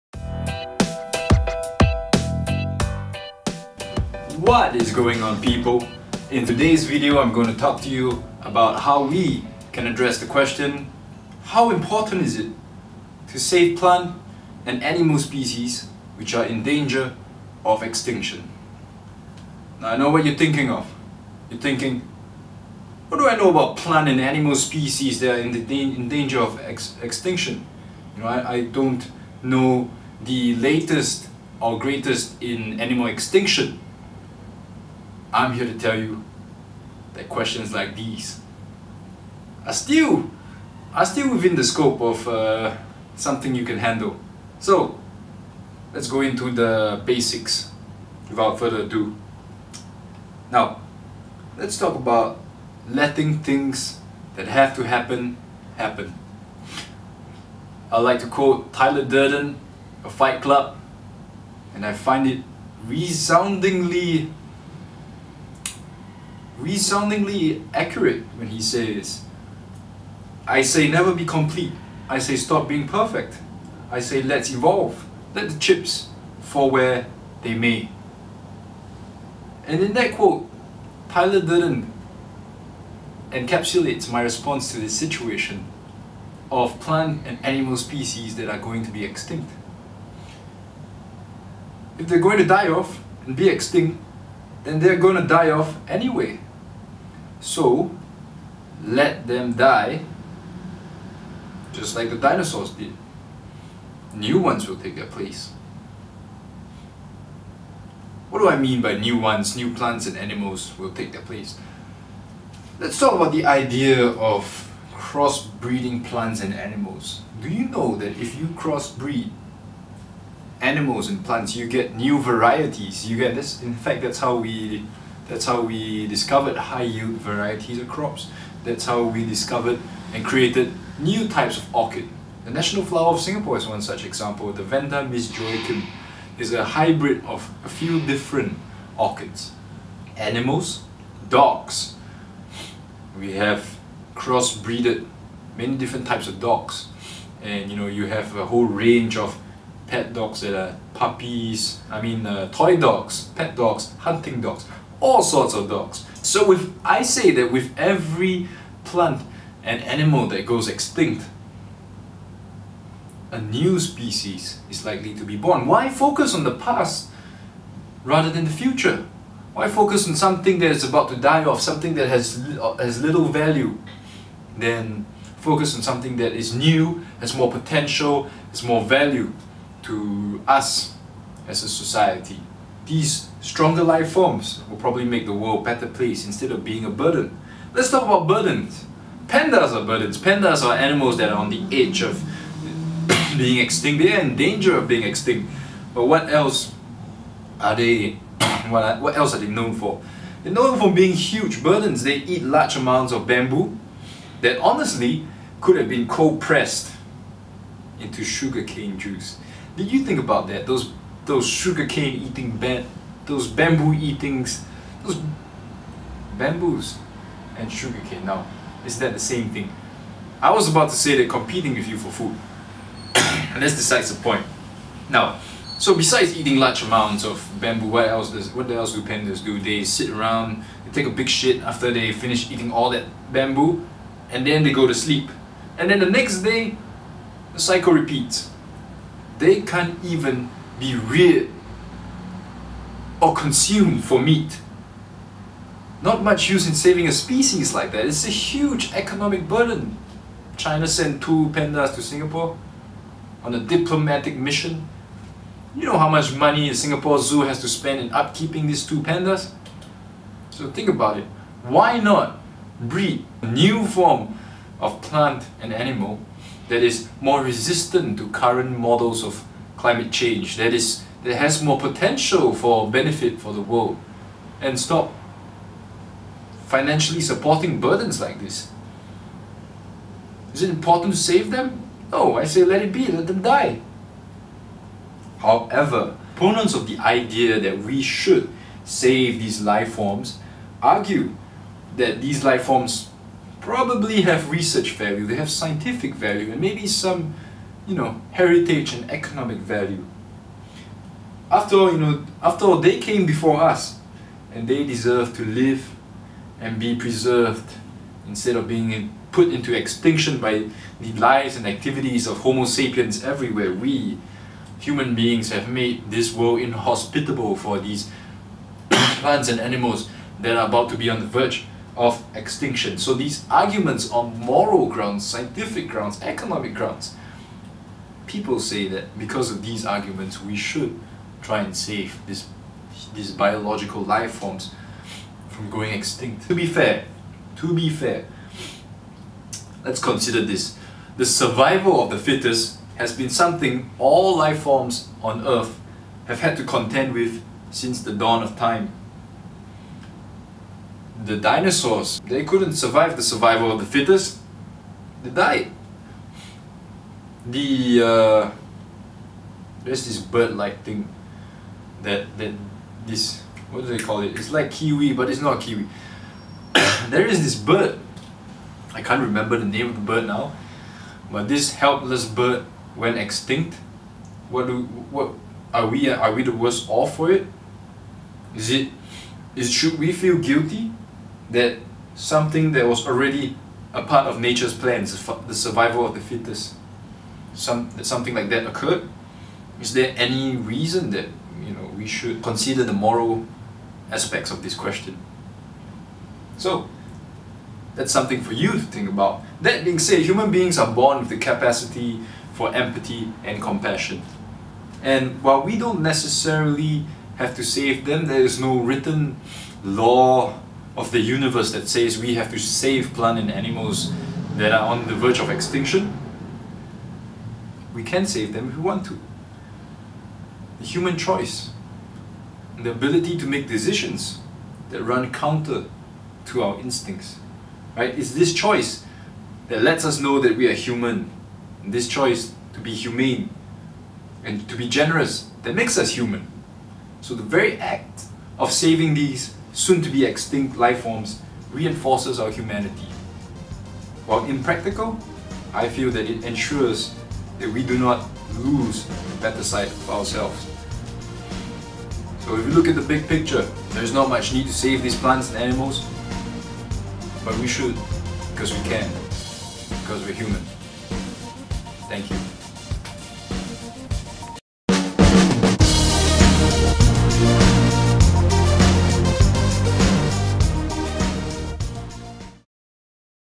6. How important is it to save plant and animal species which are in danger of extinction?
General Paper Essays on the extinction of Plants and Animals... are difficult to write?
Is this true?

Lets find out:
View the Plant and Animal Extinction Essay here!

6. How important is it to save plant and animal species which are in danger of extinction?